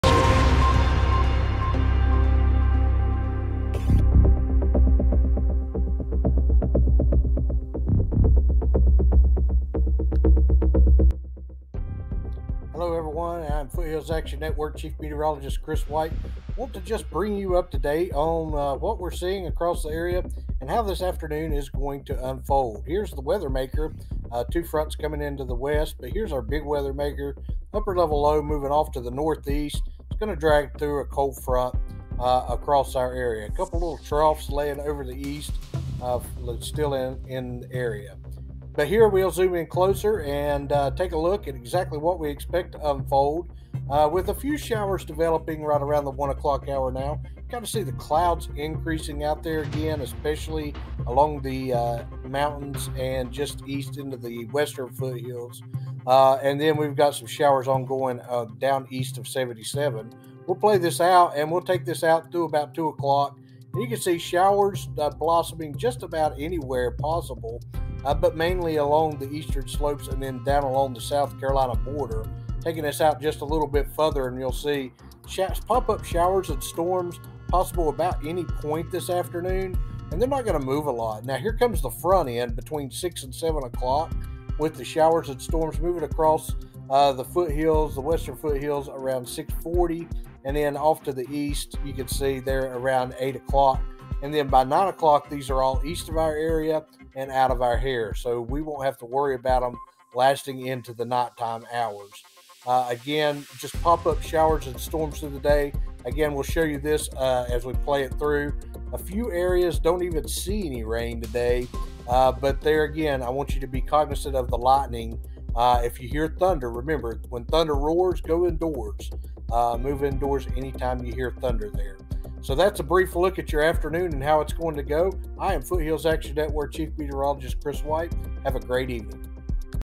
Live Sunday Afternoon Weather Update
Here is how we expect the afternoon and evening to shape up weather wise.  A few pop up storms early and then the front arriving between 6 and 8pm tonight.